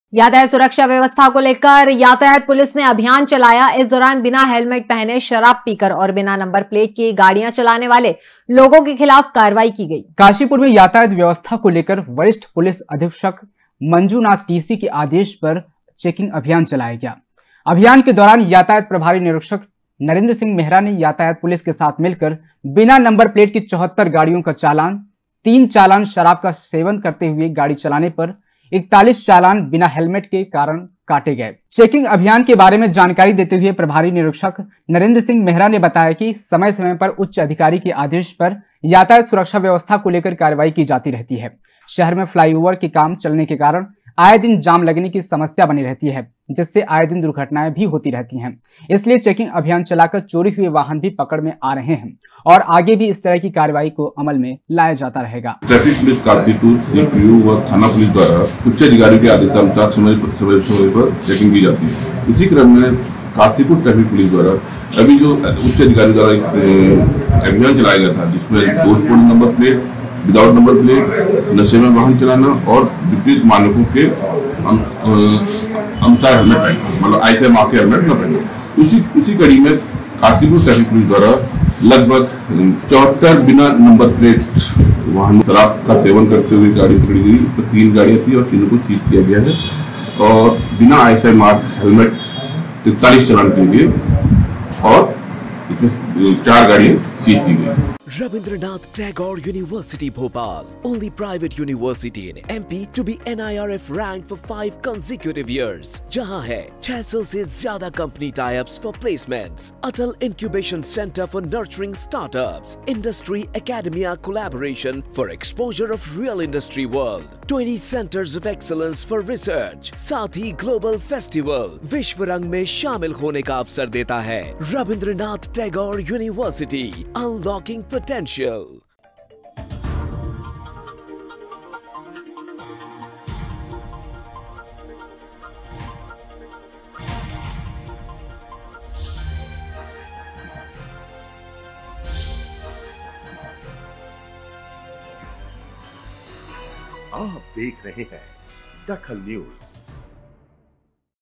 DAKHAL NEWS यातायात पुलिस ने चलाया चेकिंग अभियान
यातायात पुलिस ने चलाया चेकिंग अभियान
यातायात नियमों को तोड़ने पर कटे चालान